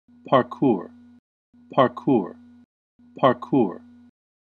How to Say or Pronounce Parkour
This video shows you how to say or pronounce Parkour.

How accurate does it say Parkour? How would you say Parkour?